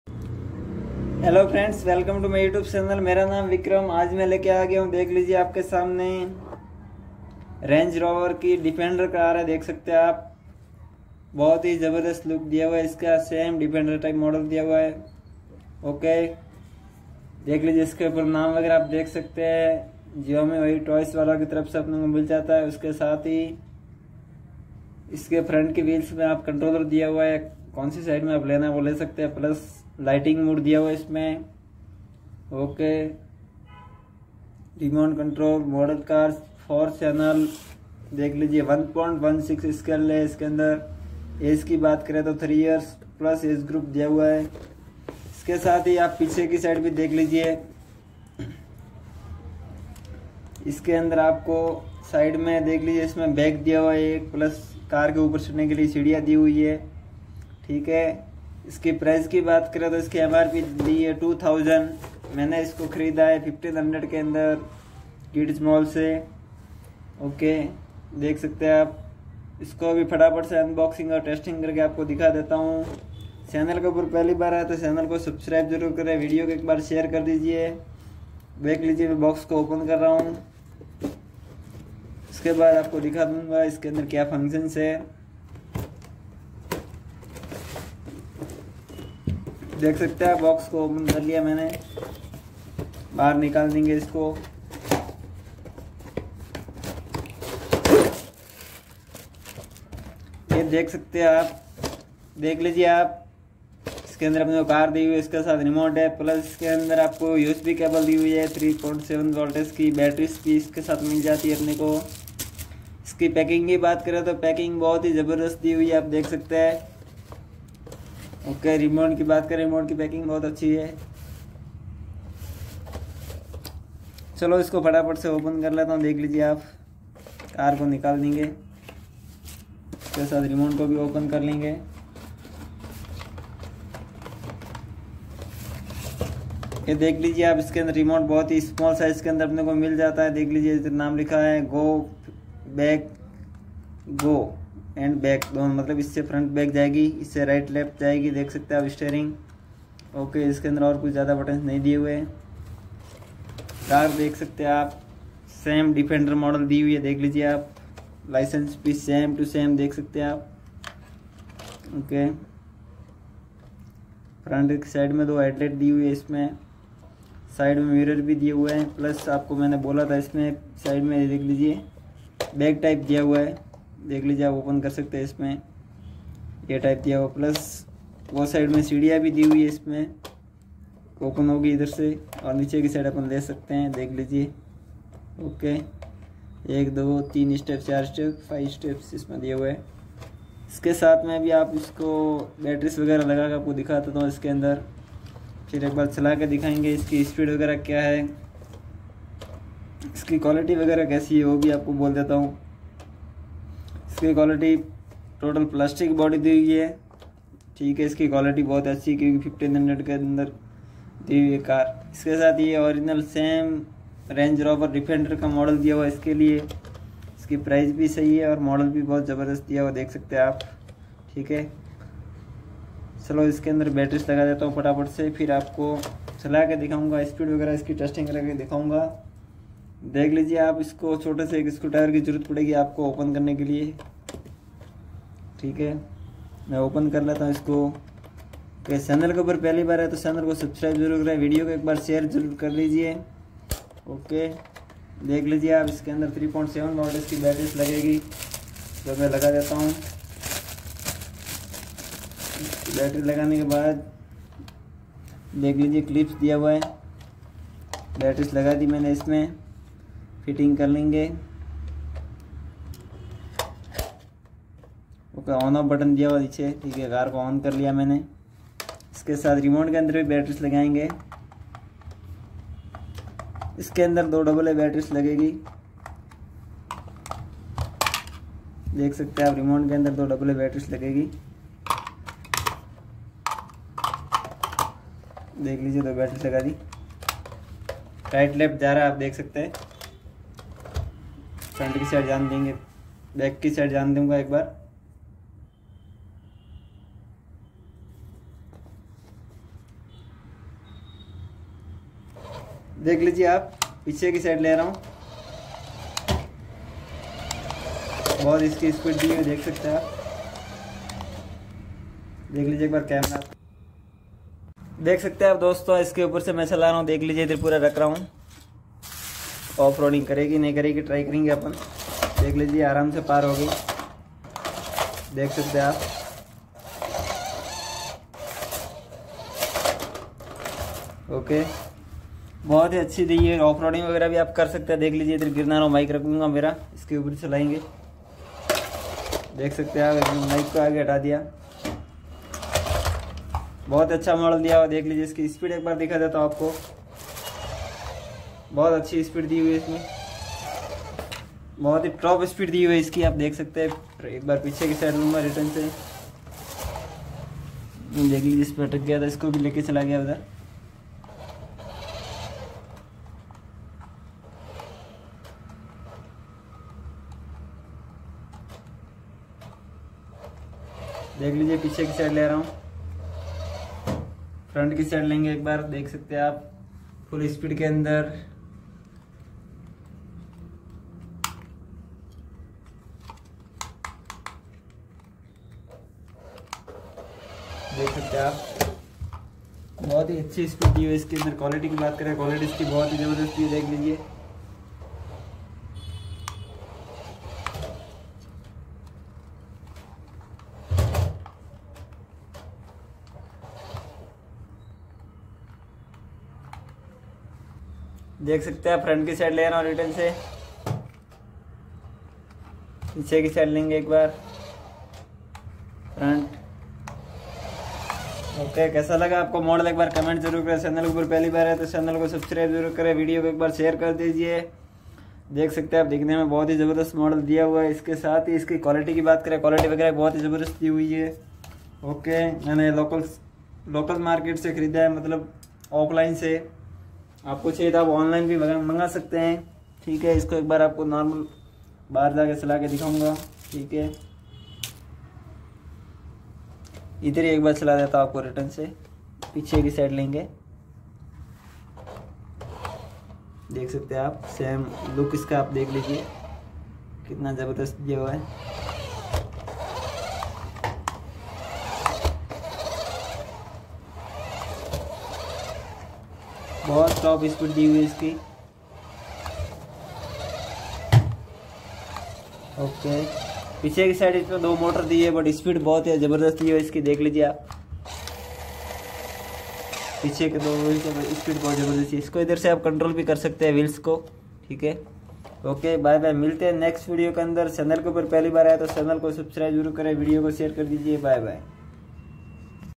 RC New 2024 LandRover Defender Car Unboxing &amp; Testing - Vikram Rajput Vlog
Dosto aaj ki video mai maine unbox aur test kiya hai rc new landrover 2024 defender car yeh.